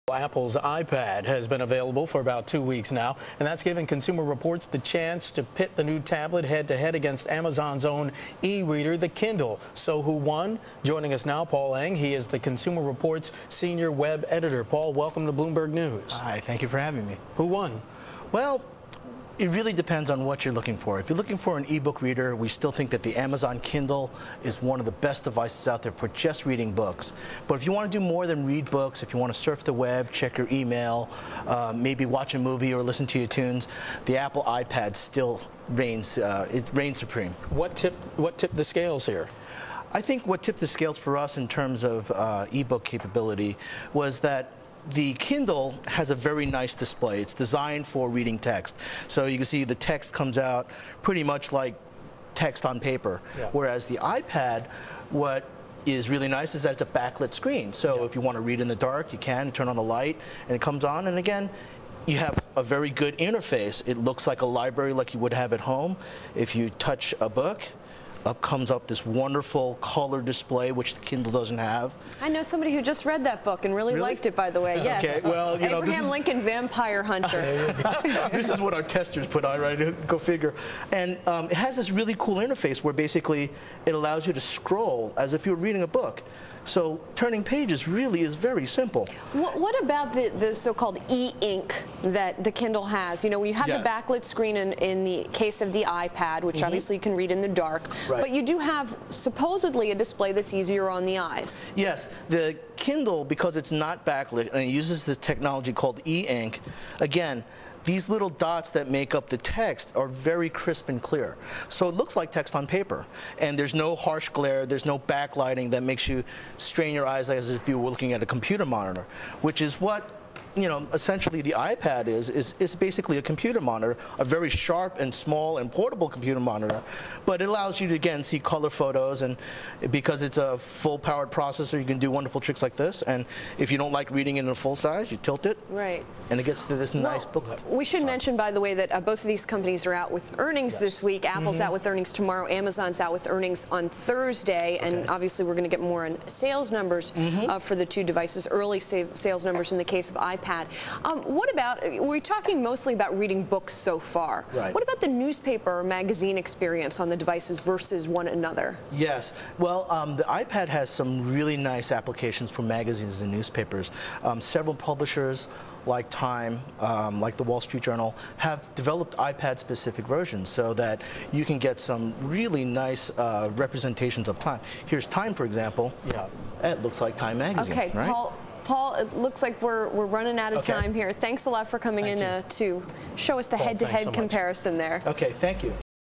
Consumer Reports' Paul Eng Compares IPad and Kindle: Video
April 19 (Bloomberg) -- Paul Eng, senior Web editor at Consumer Reports, talks with Bloomberg's Julie Hyman and Mark Crumpton about results of the magazine's comparison test of Apple Inc.'s iPad tablet computer and Amazon.com Inc.'s Kindle e-book reader. (Source: Bloomberg)